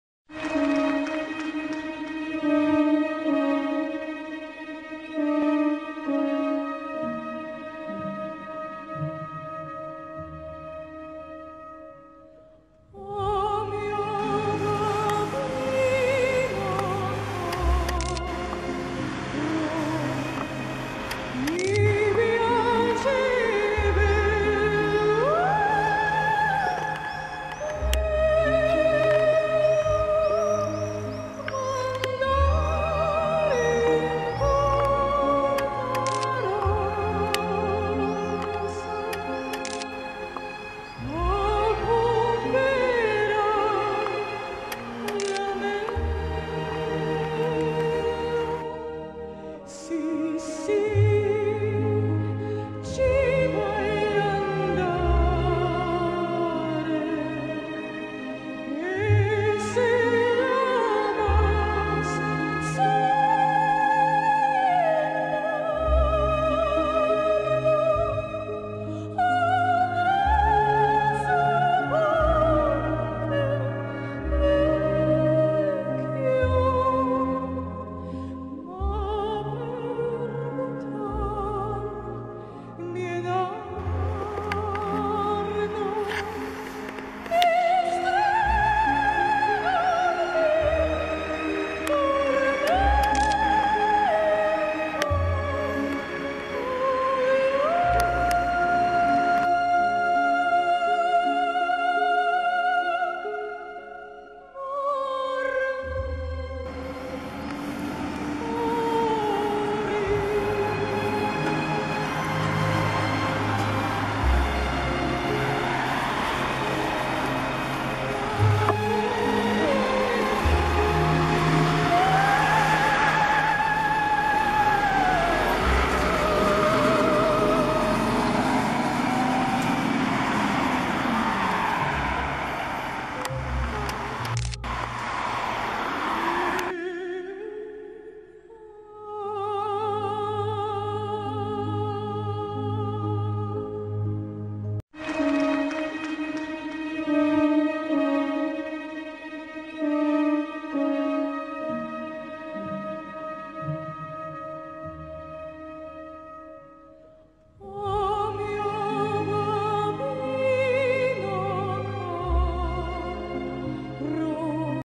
2018 07 25 Budowa Południowej Obwodnicy Warszawy cz 1 po godz 20 00
Budowa Południowej Obwodnicy Warszawy / Construction of the Southern Bypass of Warsaw / Construction de la rocade sud de Varsovie
Ulica Filipiny Płaskowickiej / Street of Filipina Płaskowicka /  Rue de Filipina Płaskowicka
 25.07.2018 po / after / après  20.00